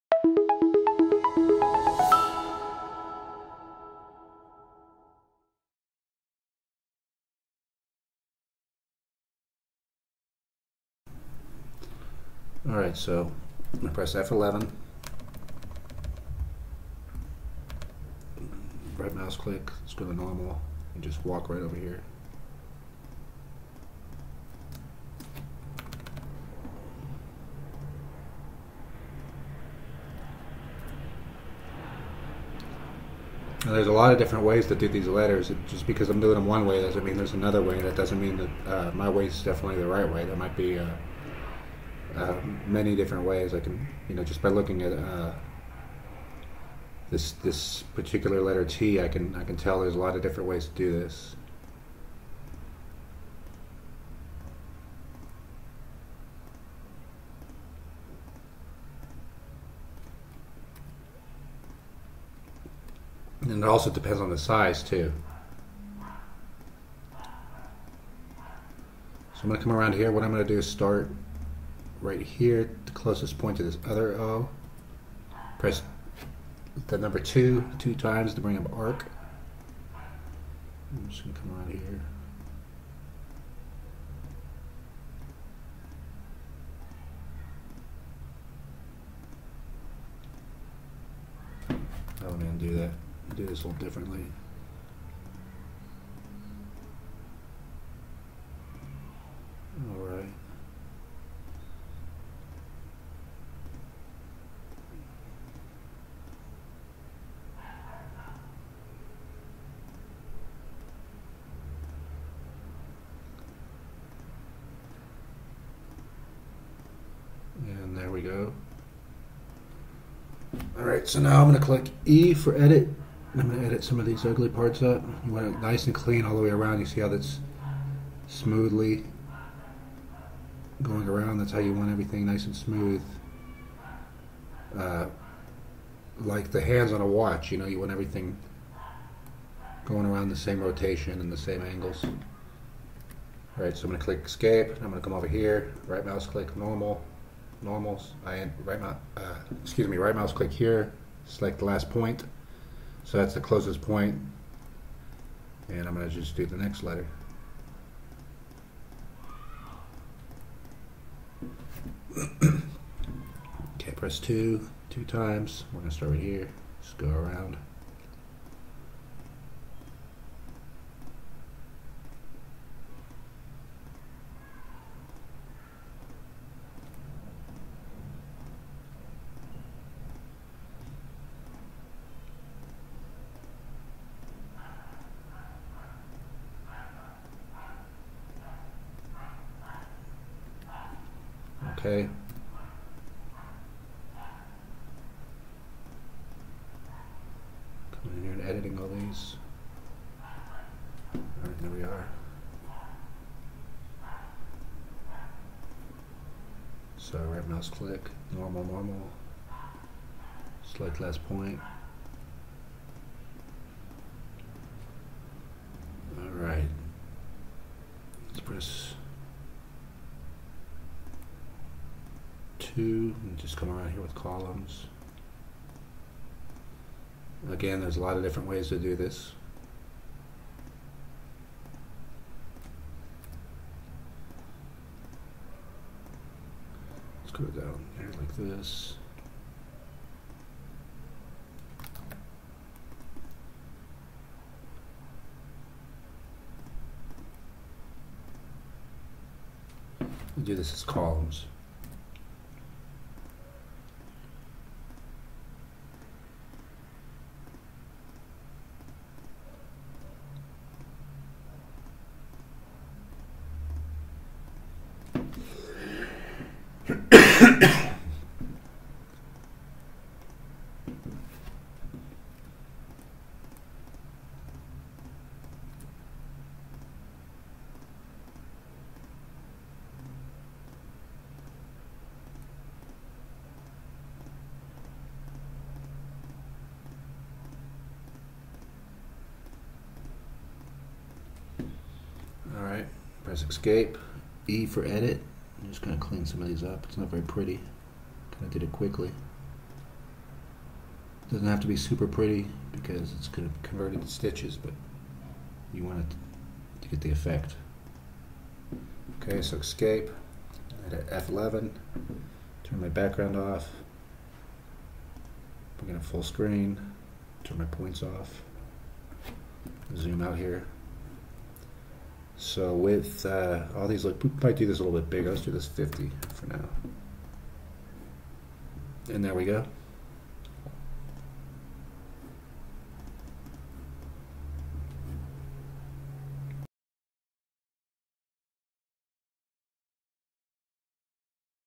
Threads Digitizing Tutorial - Trending Tools - Scissors and Text - 6 of 7
In this Threads Embroidery Software Tutorial, we show you some basic digitizing tools with an example file fro one of our customers.

In this tutorial, we show you how to digitzing a pair of scissors, and some Old English type font / text.

We also show you how to use the Mark / Block and copy and paste